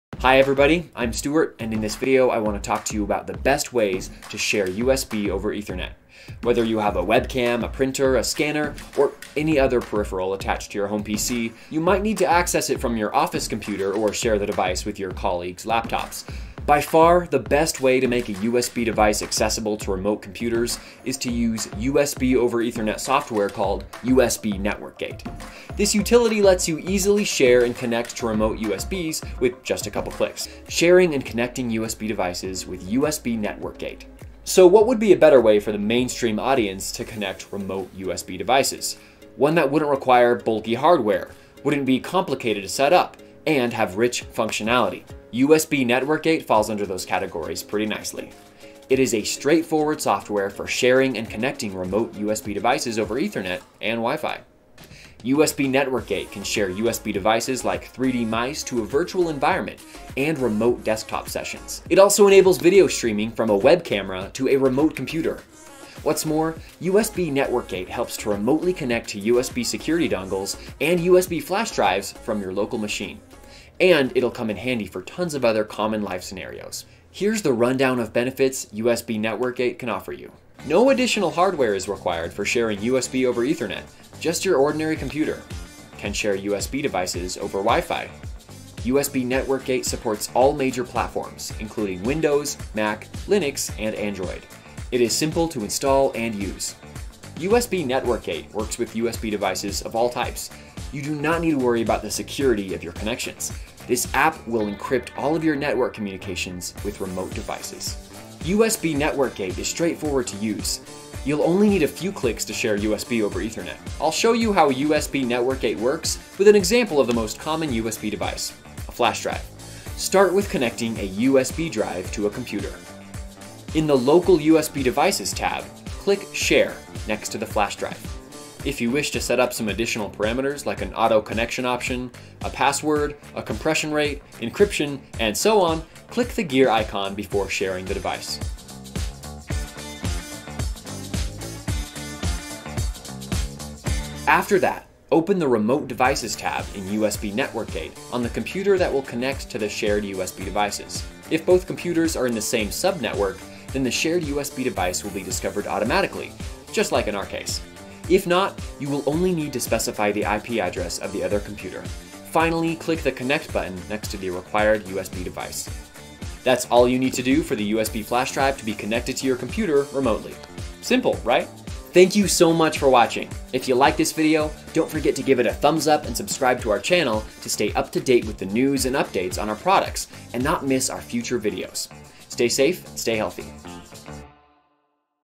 Share USB over Network. The Complete Guide to USB Network Gate
Tired of being tethered to your USB devices?  In this video, we'll introduce you to USB Network Gate, the revolutionary software that lets you share USB devices over your network – no matter where you are.

What is USB Network Gate?

USB Network Gate is the ultimate solution for accessing and sharing USB devices remotely. Whether you're working from home, collaborating with colleagues across the globe, or managing a complex industrial setup, USB Network Gate makes it seamless to share printers, scanners, cameras, 3D mice, security dongles, and more.

Unique Features & Benefits:
- Share USB over any network: Ethernet, Wi-Fi, even the Internet – it's all possible.
- Works with RDP, virtual machines, and cloud environments: access your USB devices even in the most demanding setups.
- Wide range of device compatibility: share virtually any type of USB device, including specialized industrial and medical equipment.
- Easy setup & intuitive interface: get up and running quickly with our user-friendly software.
- Secure & reliable: advanced security features protect your data and ensure stable connections.

Why Choose USB Network Gate?

If you need to access USB devices remotely, collaborate on projects that require specialized hardware, or simply want to expand the reach of your USB peripherals, USB Network Gate is the answer. It's the perfect tool for individuals, teams, and businesses of all sizes.

Intro: (0:00)
USB over Network software: (0:30)
Usage scenarios: (0:55)
USB Network Gate benefits: (1:20)
How to share USB over network: (1:54)
Outro: (2:52)